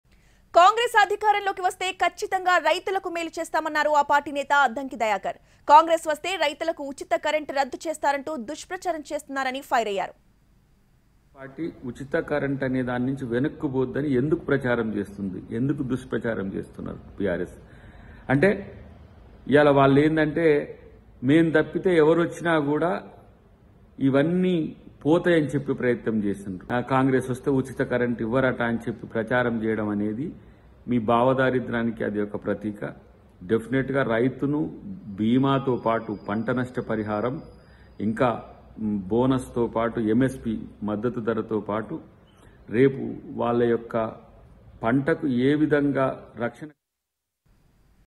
Addanki Dayakar Serious On BRS Leaders Comments | Revanth Reddy | Ntv
For more latest updates on the news :
Addanki Dayakar Fires On BRS Leaders Comments | Revanth Reddy | Ntv